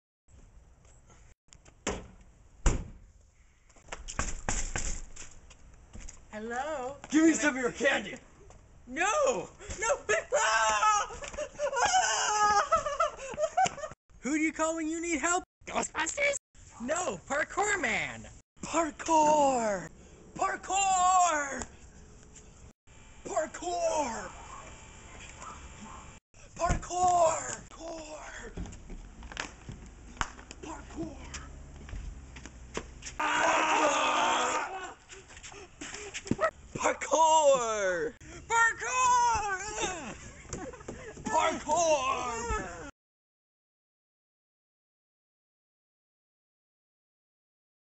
Man Saved From Bully By Parkour!
I was at my front door and this guy said, "Give me your candy" and starting beating me with a hammer.  I didn't know what to do until Parkour man came.